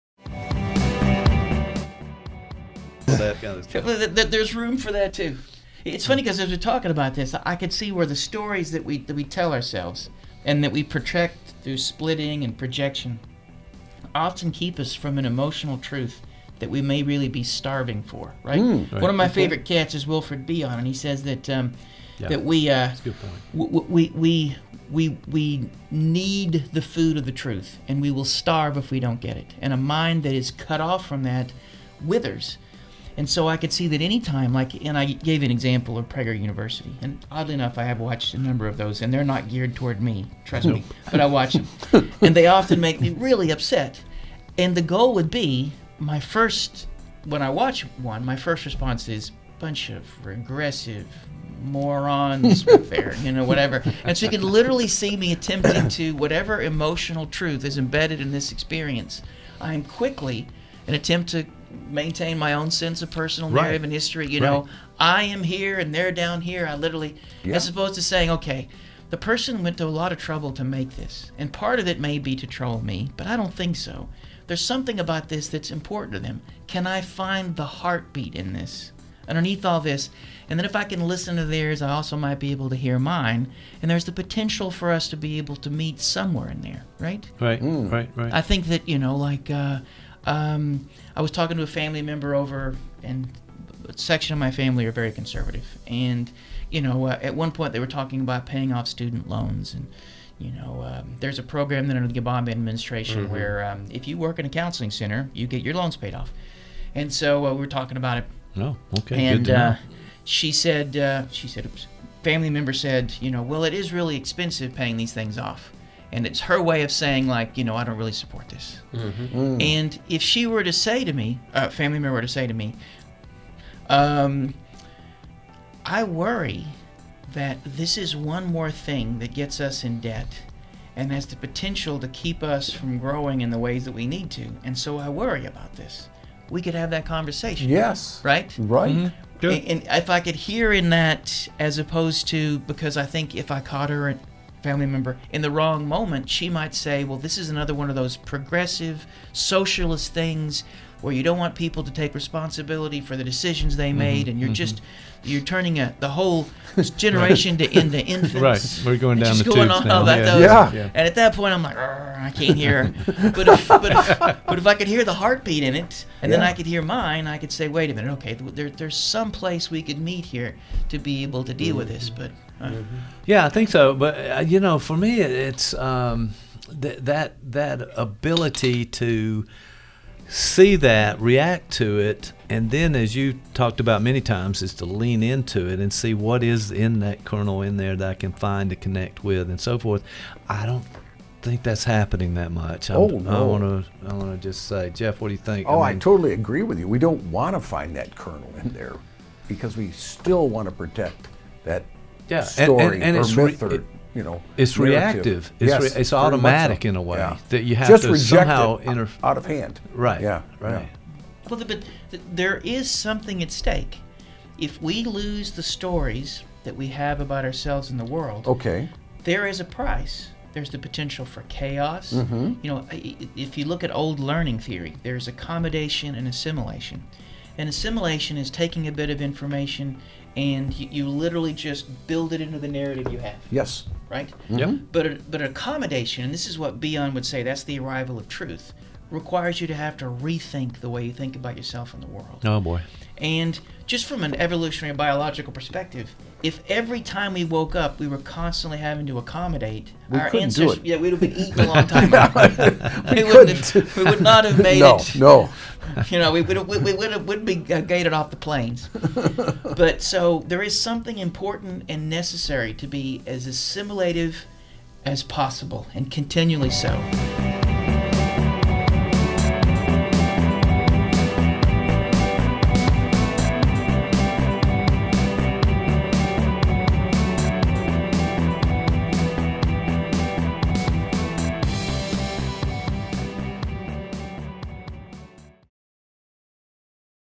Assimilation and Accomodation
Discussion around assimilation and Accomodation on the Gang Podcast of Got Therapy?

also visit our YouTube channel: Columbus Television